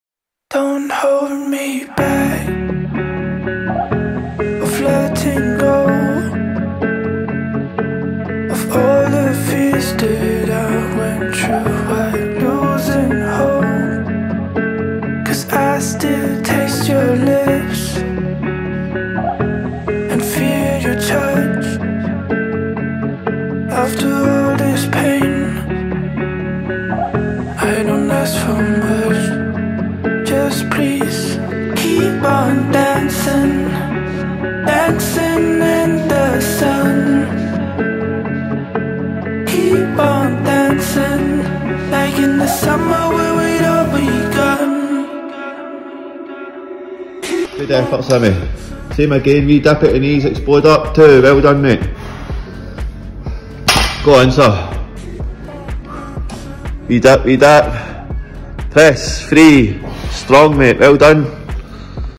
Stephen S-PT-Session 30
Simmy with a heavy set ,pre fatigue session,great effort and will def be tender after it 🔥👍🏻💪🏻